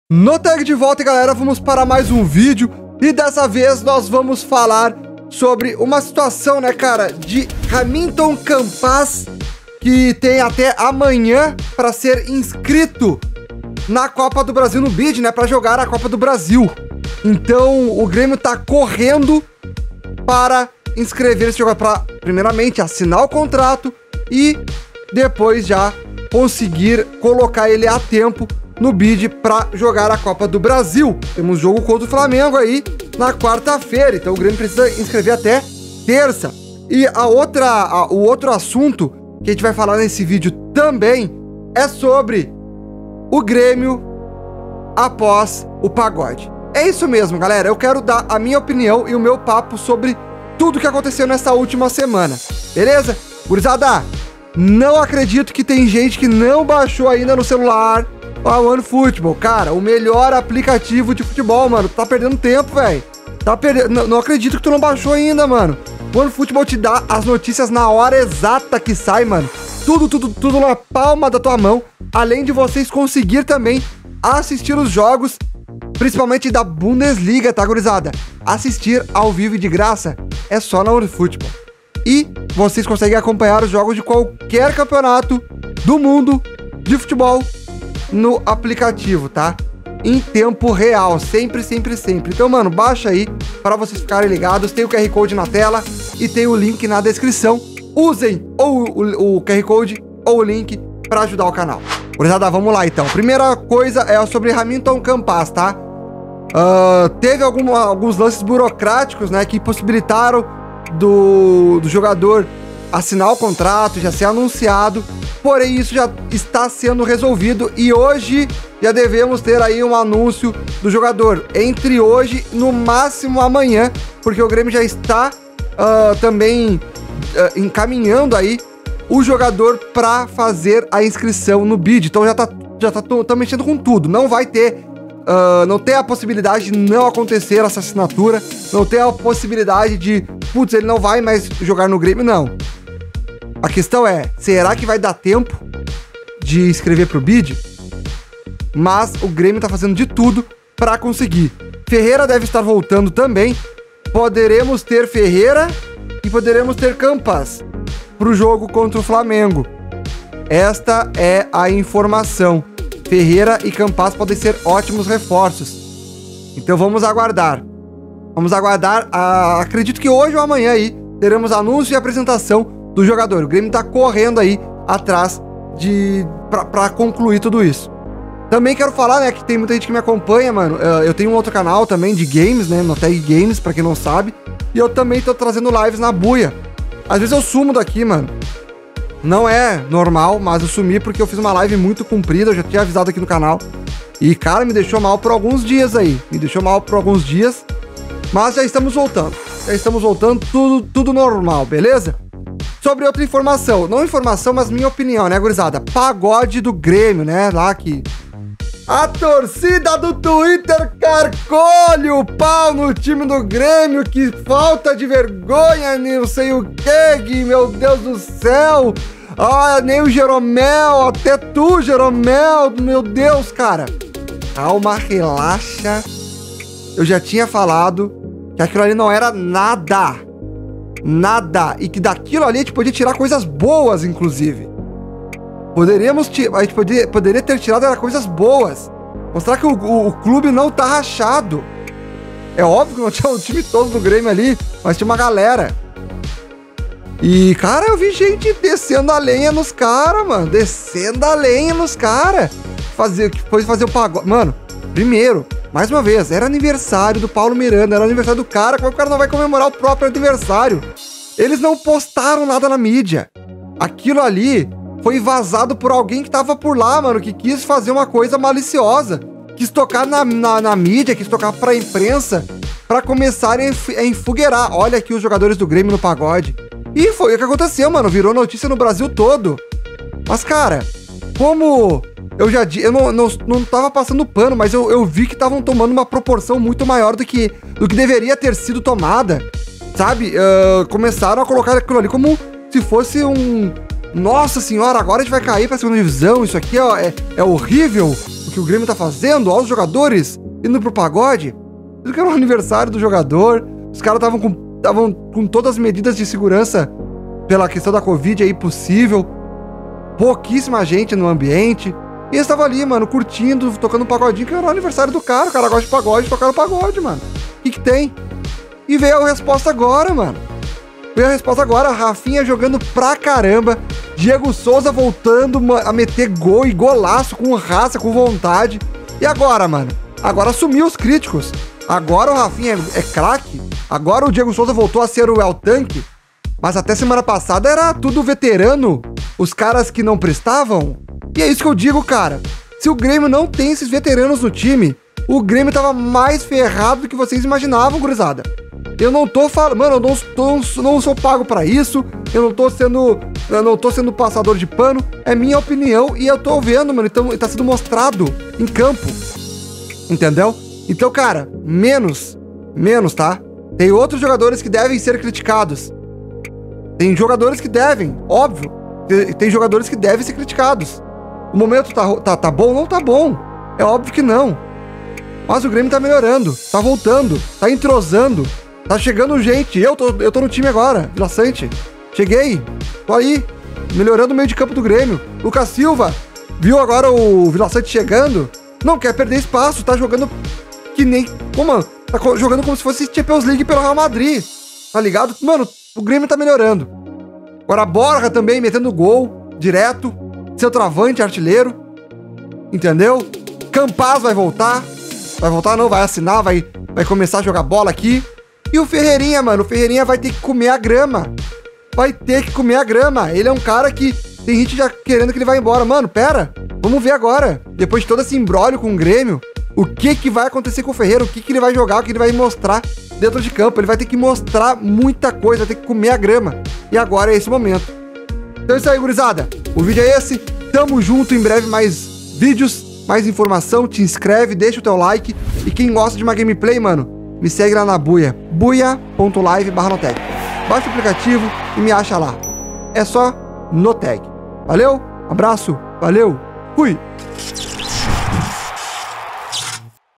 INFORMAÇÃO SOBRE CAMPAZ, ANÚNCIO E QUANDO JOGA! E O PAGODE? AJUDOU O TIME!
Campaz deve ter anúncio em breve e pode jogar contra Flamengo!
E o pagode do Grêmio ajudou?